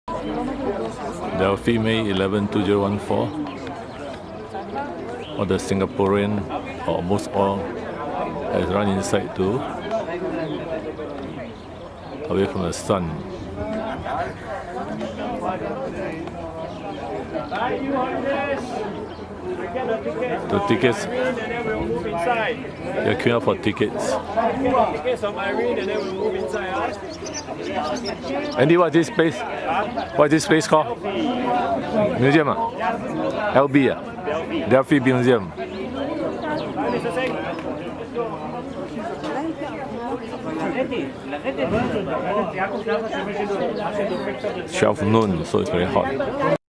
Delphi Museum, Greece
May 11, 2014. Noon time. Hot, hot, hot forl the fair ladies of Singapore. Brought their umbrellas too. Hats and sun-screen important.